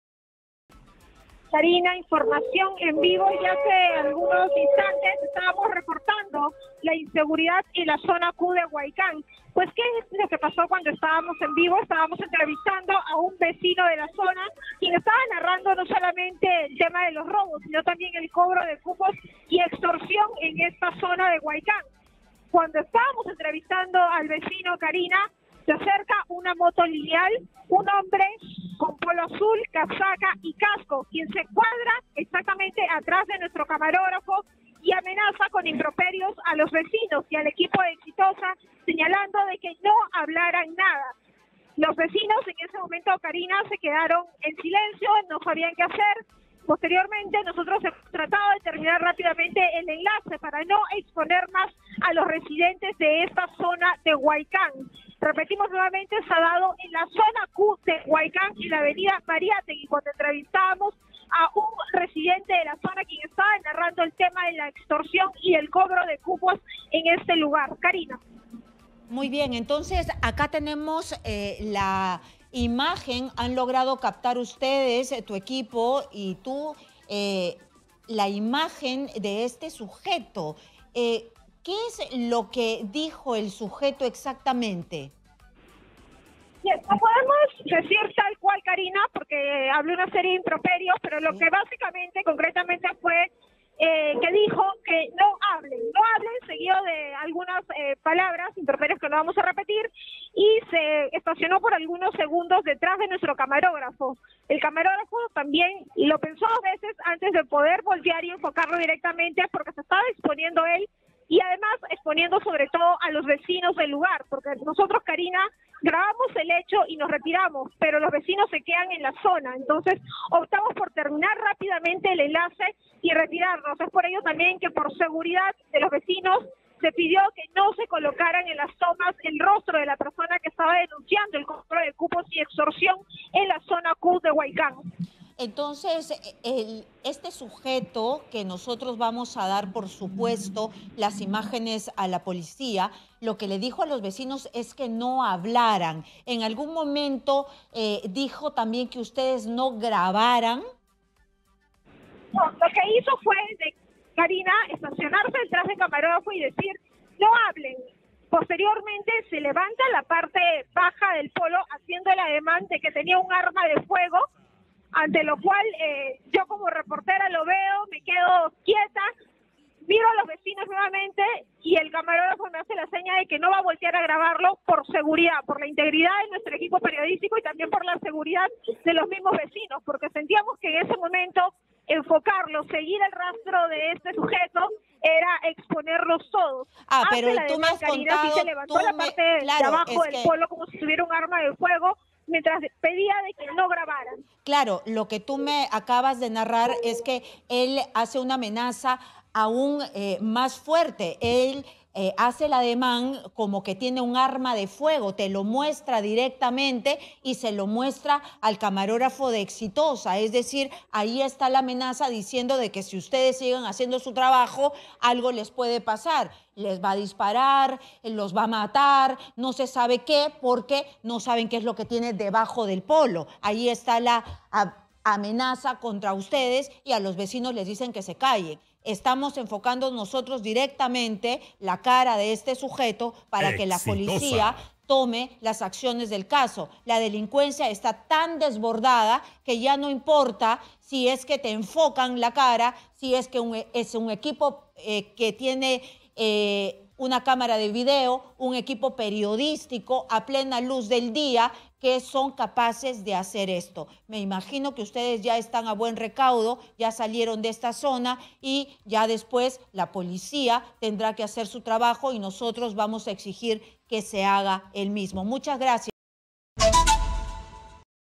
🔴🔵 Huaycán: Equipo de Equipo de Exitosa y vecinos fueron amenazados por motorizados
Huaycán: Reportera de Exitosa comenta cómo el motorizado se acercó para amenazarlos.

Exitosa Perú
"Exitosa: La voz que integra al Perú"

Corporación Universal